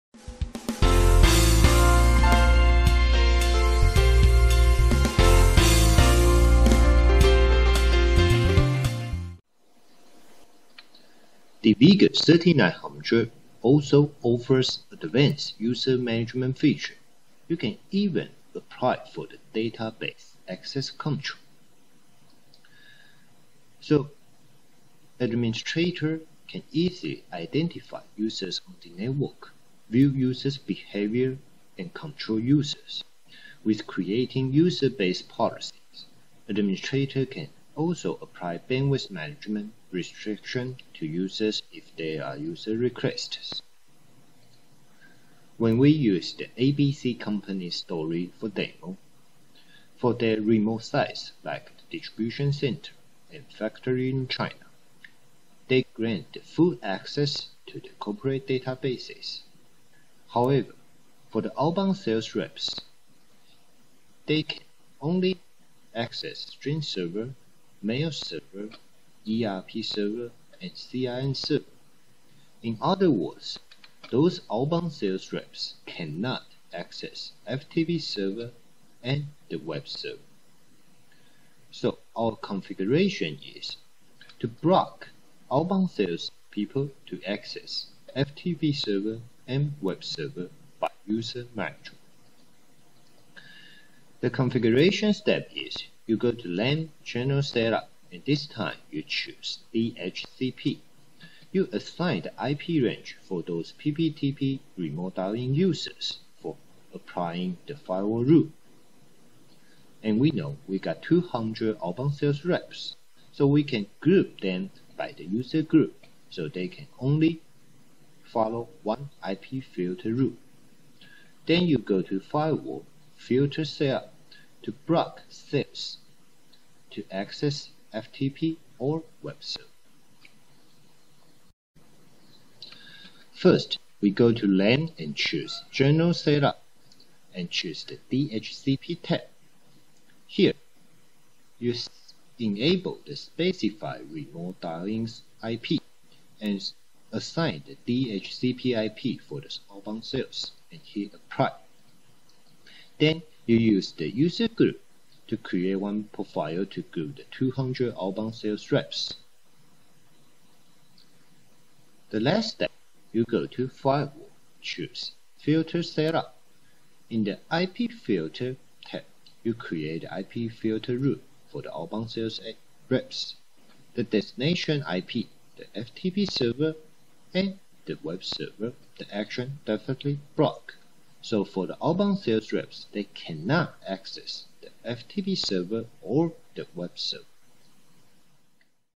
DrayTek Vigor3900 - User MGMT
The user management feature allows the administrator to authorize different access right to different users/user groups. Your confidential data won't be viewed by unauthorized personals.